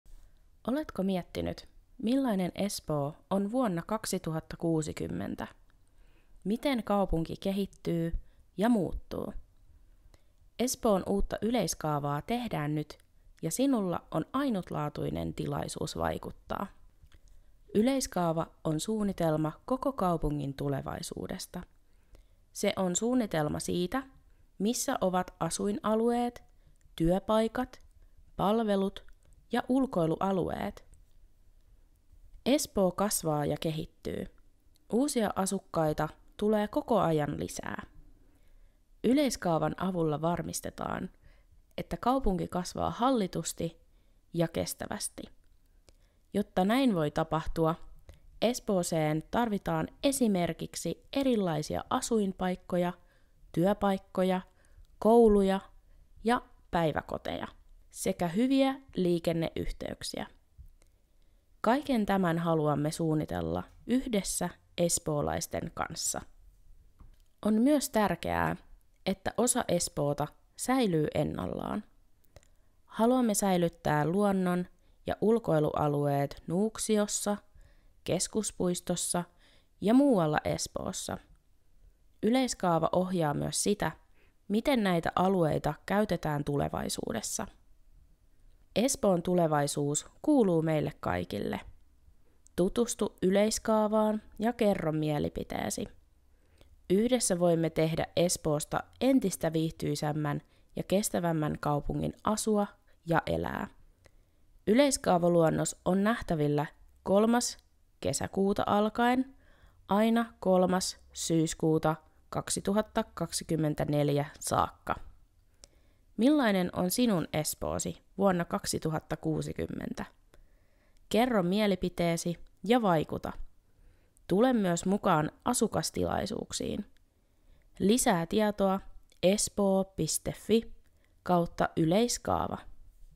Espoon yleiskaava 2060 selkeällä kielellä
Nyt on aika vaikuttaa! Espoon yleiskaava 2060 luonnos on nähtävillä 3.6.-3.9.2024. Tule mukaan asukasiltoihin